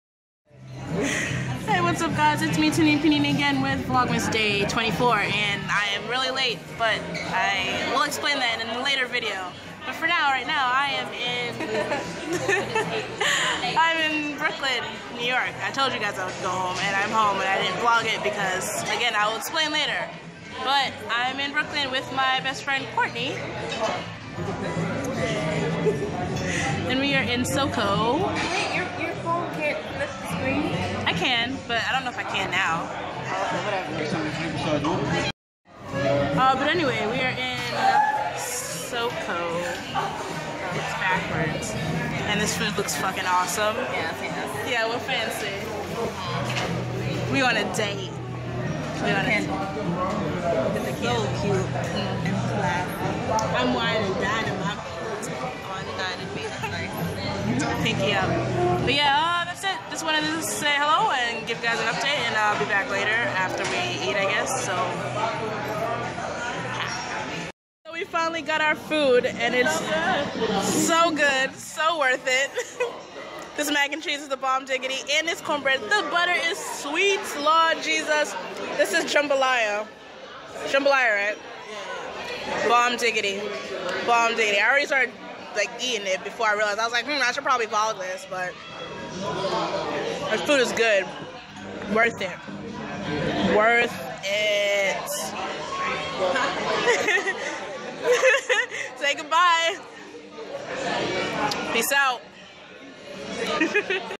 Vlogmas Day 24- Christmas EVE!
I'm back in NYC!
I missed home.
I hope you enjoyed, even if it's a bit short.

Love Ya!